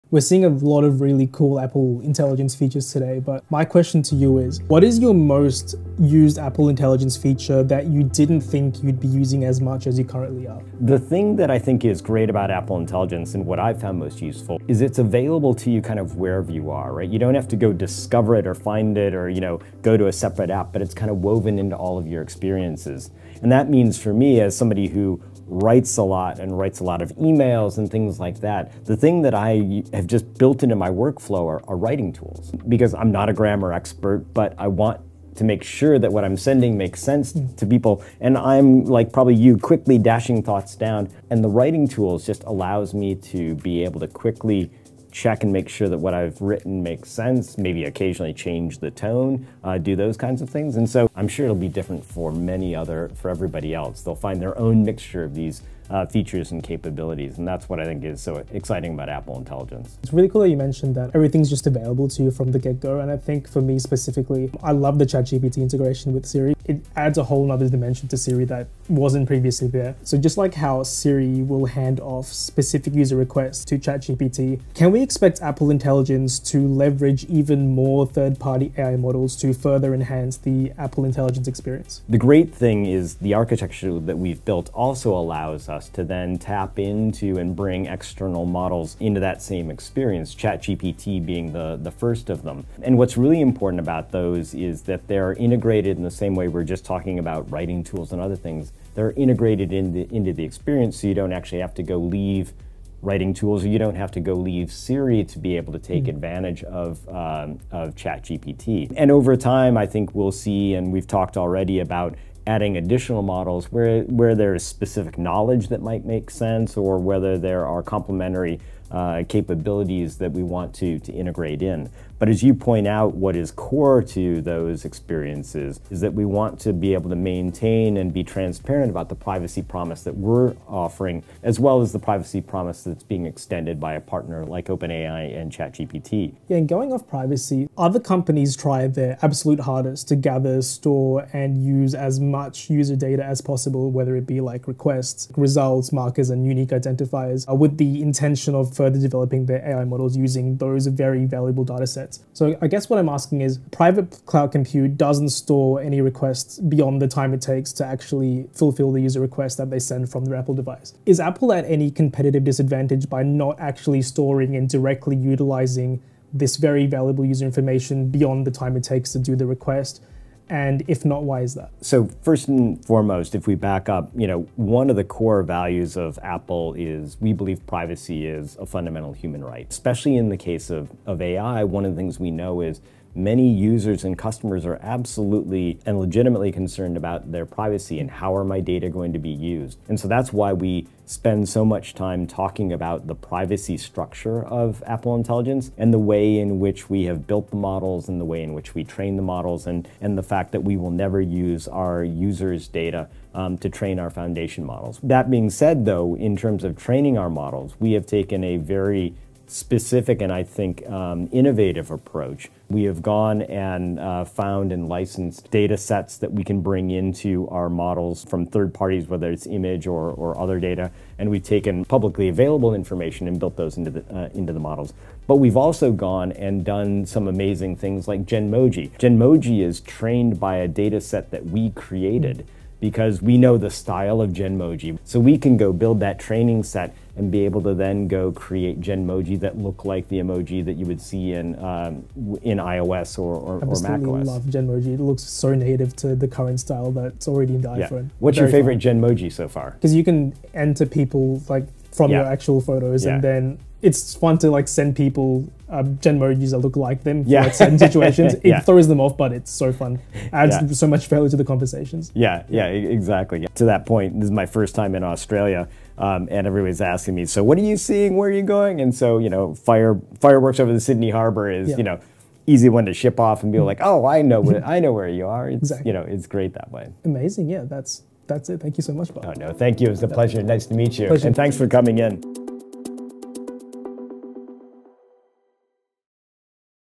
Is Apple Intelligence ACTUALLY private? | Chatting AI & Privacy with Apple VP Bob Borchers
I sat down with Bob Borchers, Apple's vice president of worldwide product marketing, and discussed the ins and outs of Apple Intelligence, especially the privacy frameworks at play, following the launch of iOS 18.2 and consequent launch of many Apple Intelligence features.